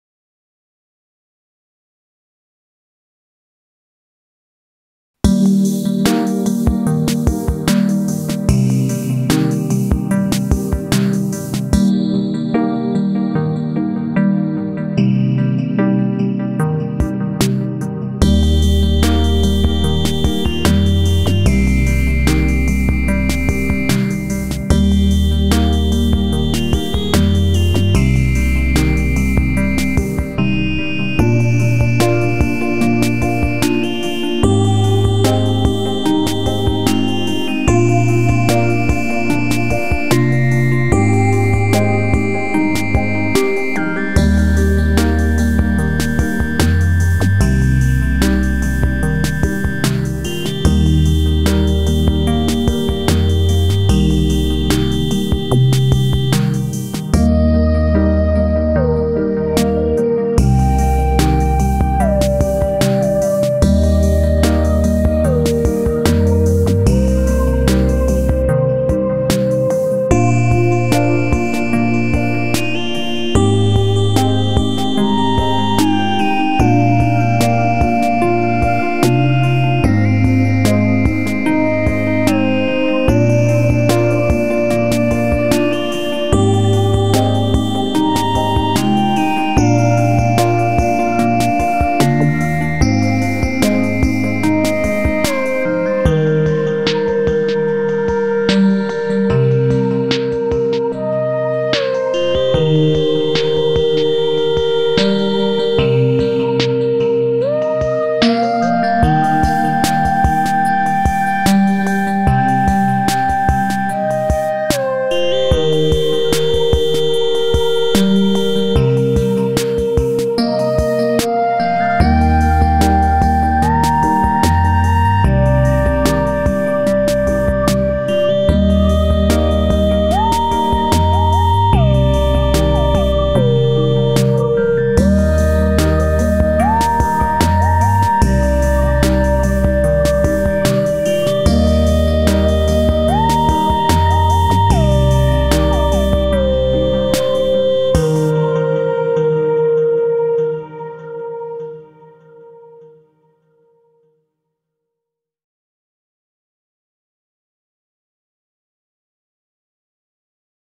The Ringed Ruins Of Moray, Peru
The ancient site of Moray, Peru, is located across from the Sacred Valley of the Incas, 26 miles (42 km) from Machu Picchu.  It consists of sets of concentric terraced circles that form two amphitheaters. Theories are that it was used for agricultural purposes, it was a spiritual temple, or that it was used for astronomical purposes.  I say all of the above.